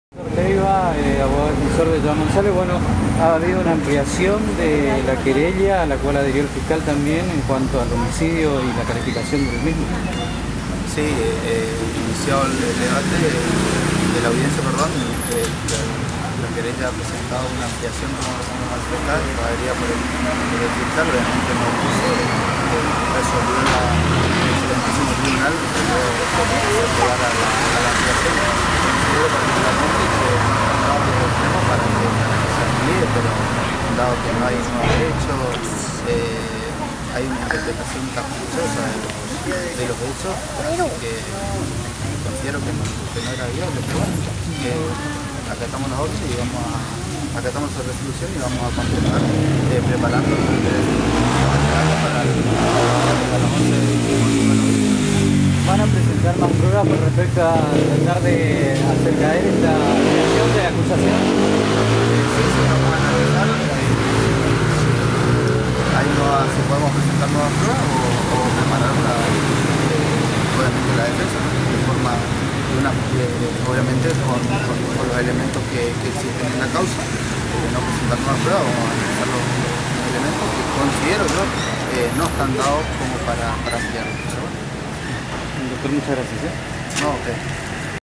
Dr Martin Leiva Abogado De Joan Gonzalez.-
| Juicio a Joan Gonzalez por el asesinato de Ivan Acuña.